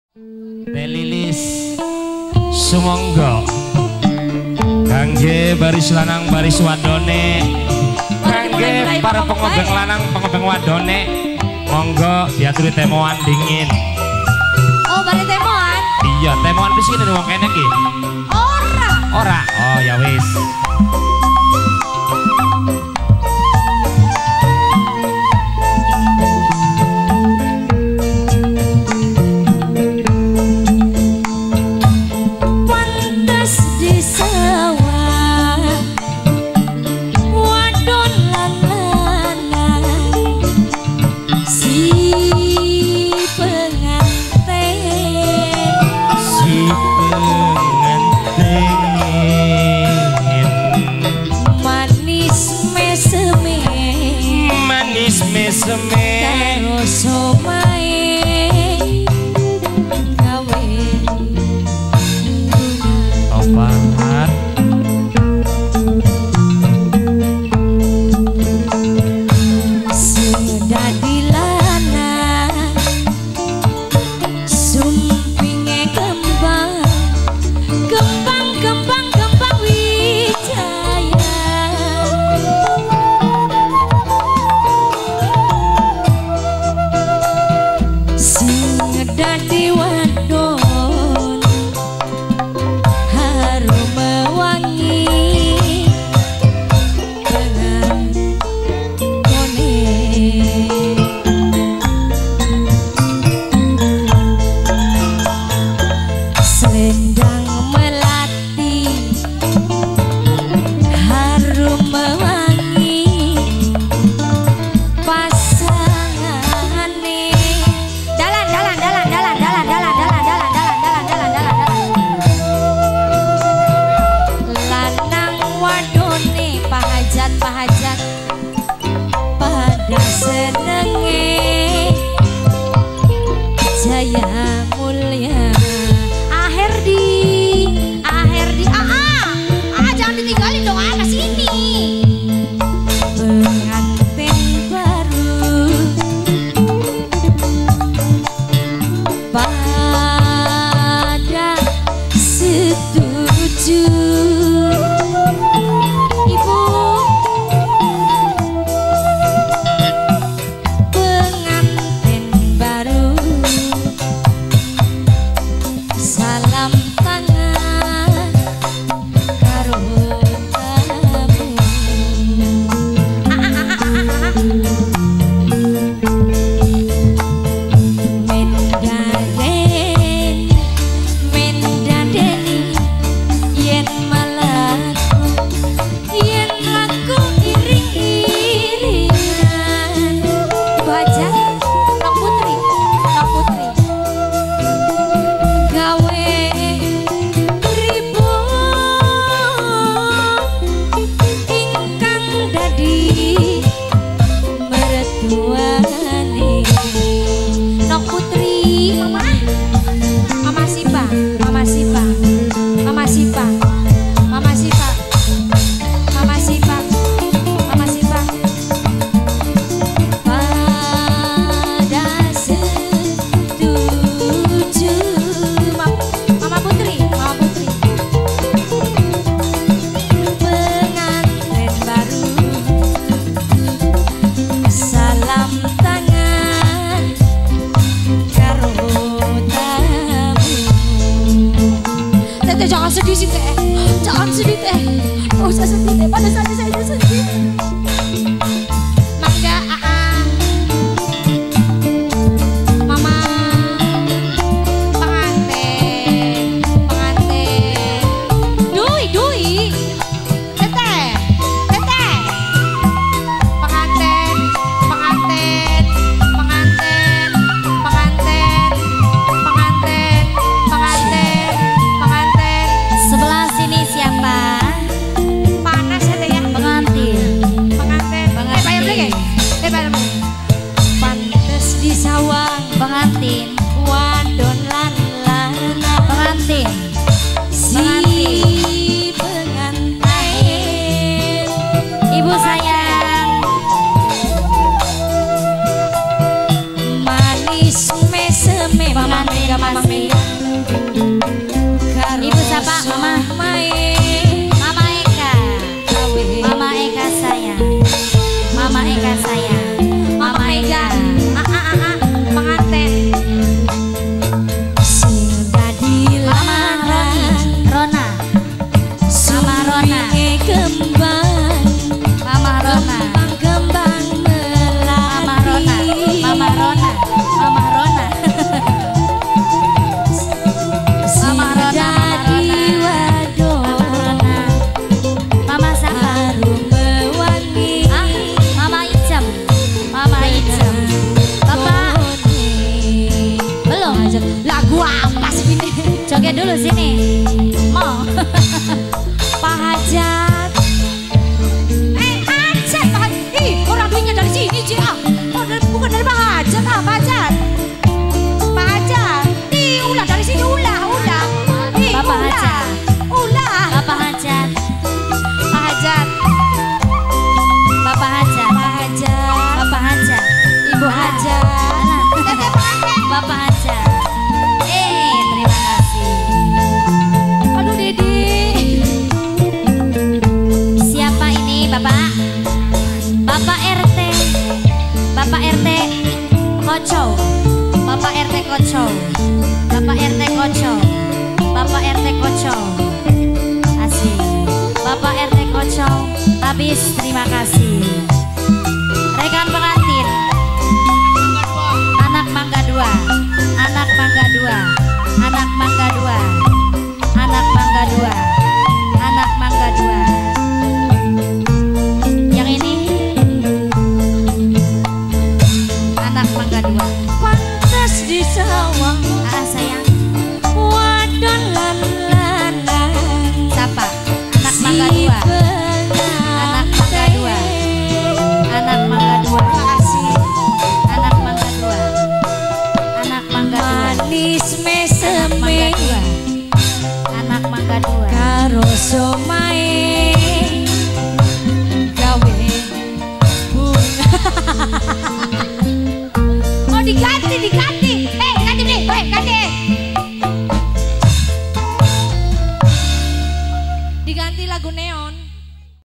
10 THE GREAT OF TARLING DANGDUT MAHAJAYA @ Kertamulya Mekarjaya Kertajati Majalengka
Desa Mekarjaya 20 Juni 2019

Pernikahan
Lia Rosliana & Herdi Setiawan

Gusaran
Putri Fauziah

Keluarga Hajat
Bpk. Rasmadi & Ibu. Lilis Rohaeni
Bpk. Seki (Alm) & Ibu. Rayem (Almh)
Bpk. Tarham (Alm) & Ibu. Titin

Akad Nikah
Dsn. Kertamulya Ds. Mekarjaya Kec. Kertajati Kab. Majalengka

THE GREAT OF TARLING DANGDUT
MAHAJAYA
Pimp. Ang Prilit Cs